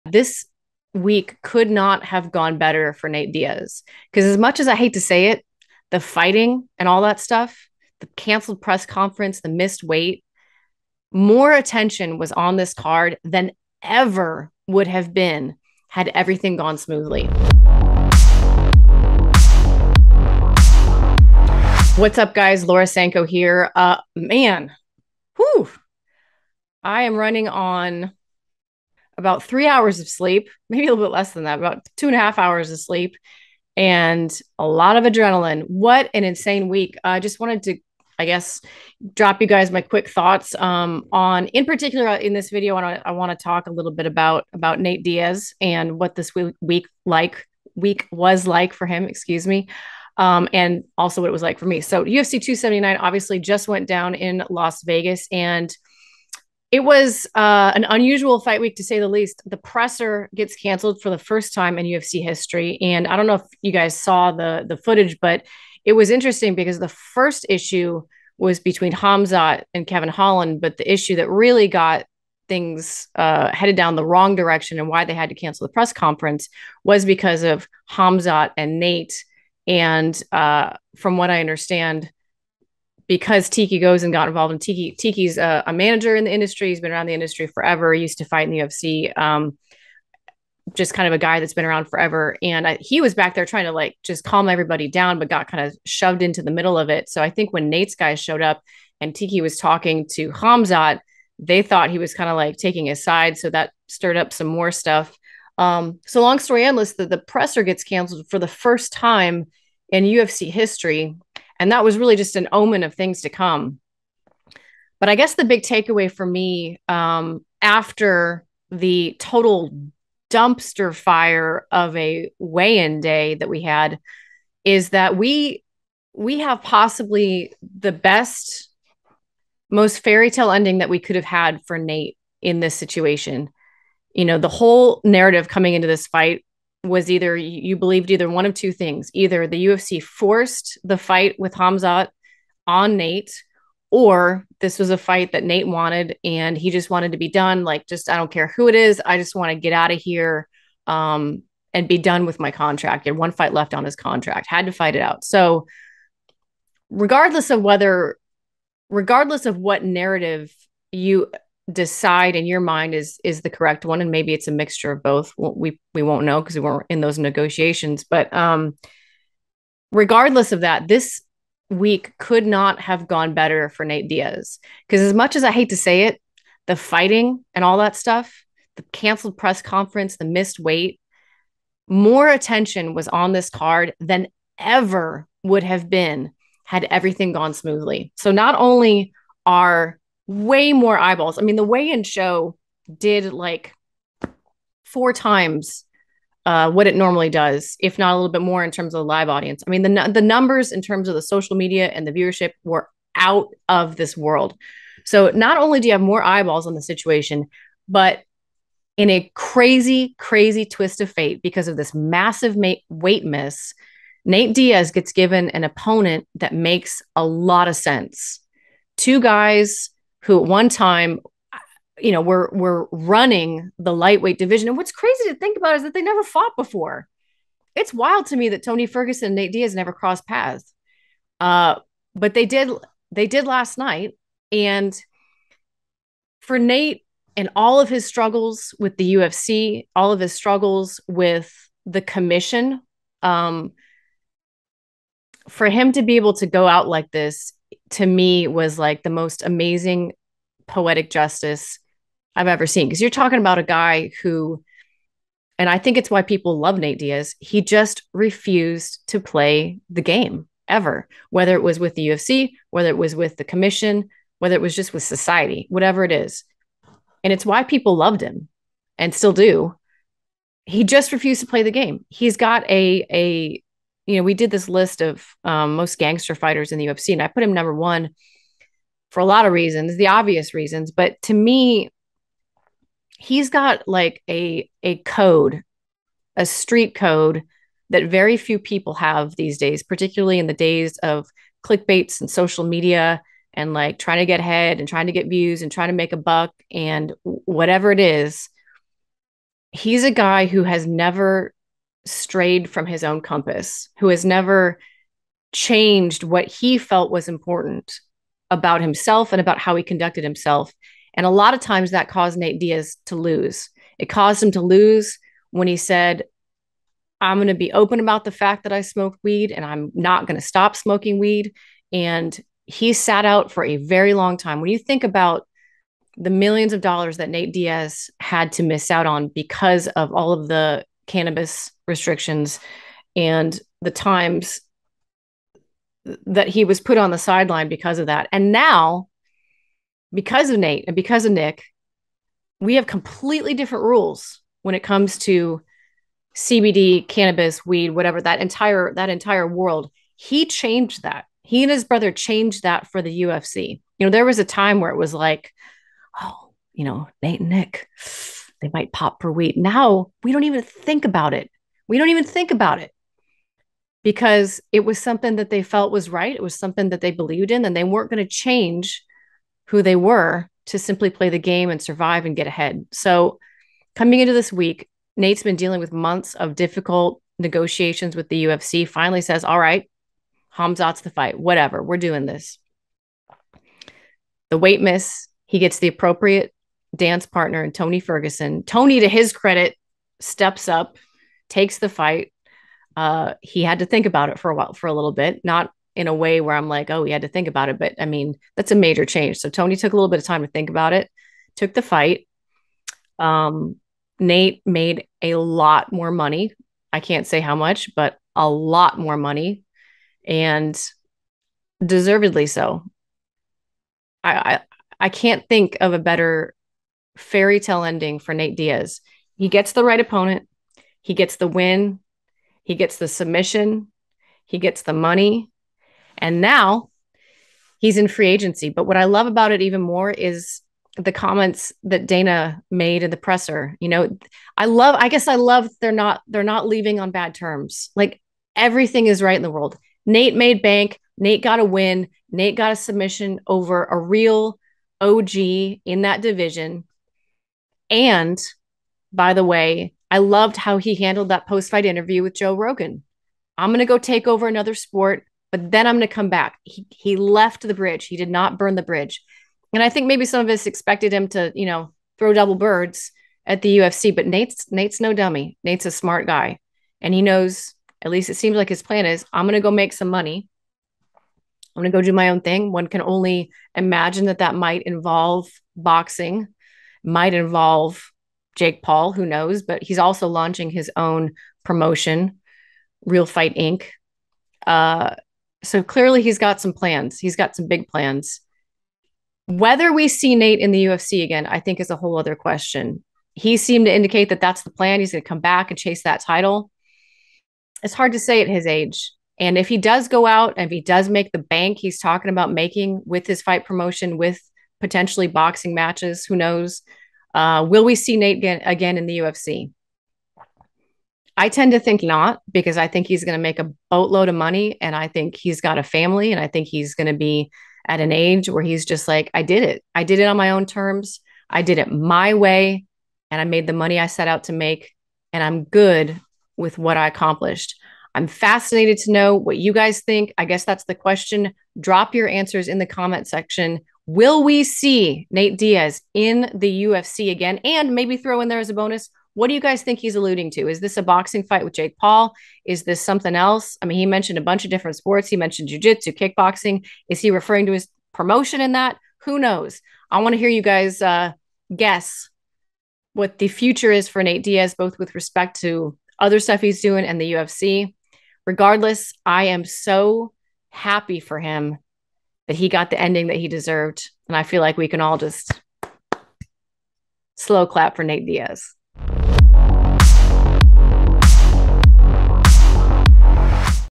"A Fairy-Tale Ending For Diaz" - Laura Sanko on UFC 279
Laura Sanko shares her thoughts on Nate Diaz's last bout, the excitement of how the event unfolded and what's next for him?

Catch us on twitter @1on1 where we will be posting regular MMA content and hosting giveaways!